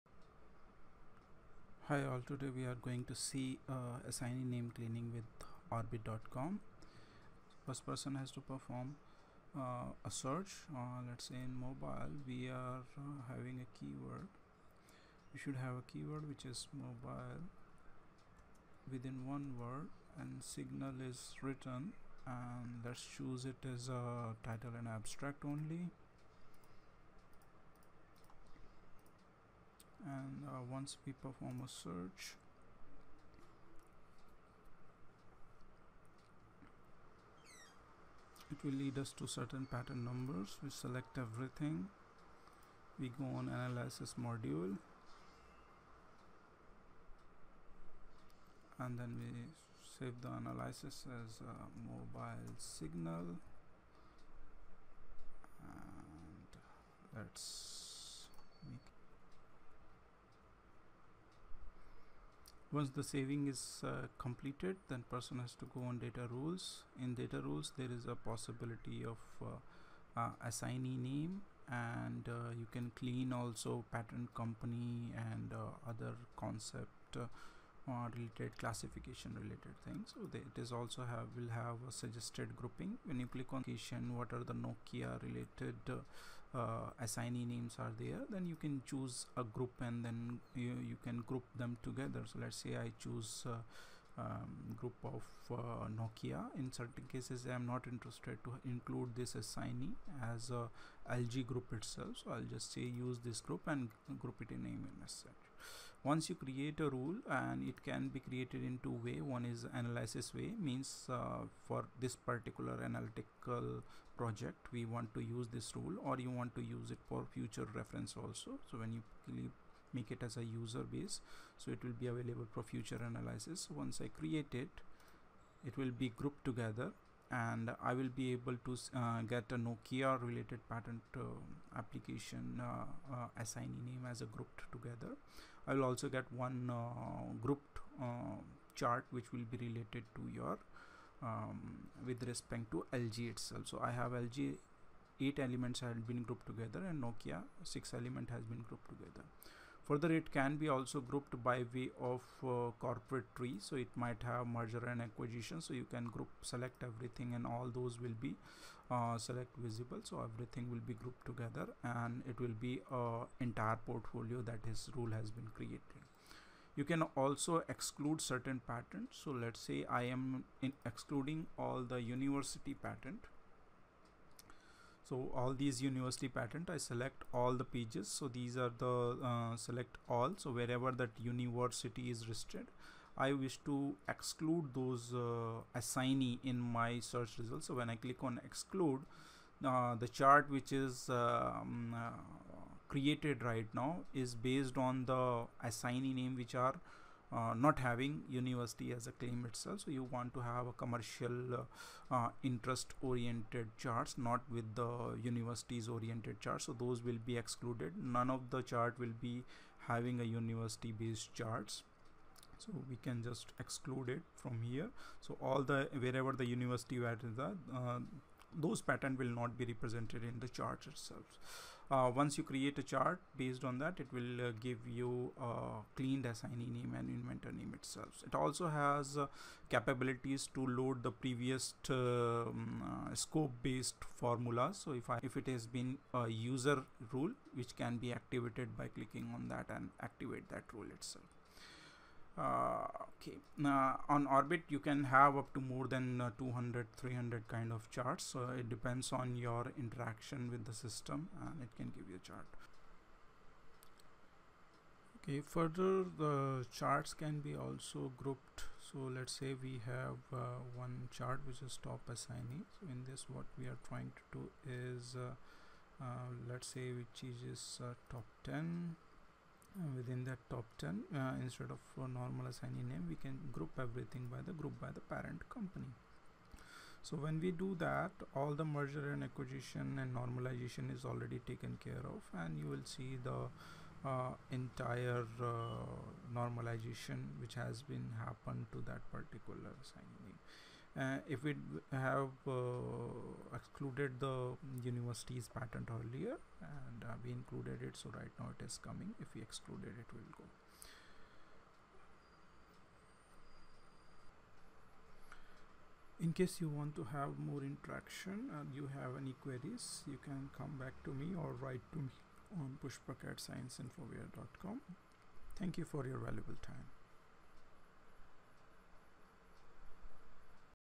Questel Orbit Assignee Based Charts
How to use Questel Orbit for Assignee name Normalization and Group, Mergers, Parent Companies, Group by Supplier, Competitors etc.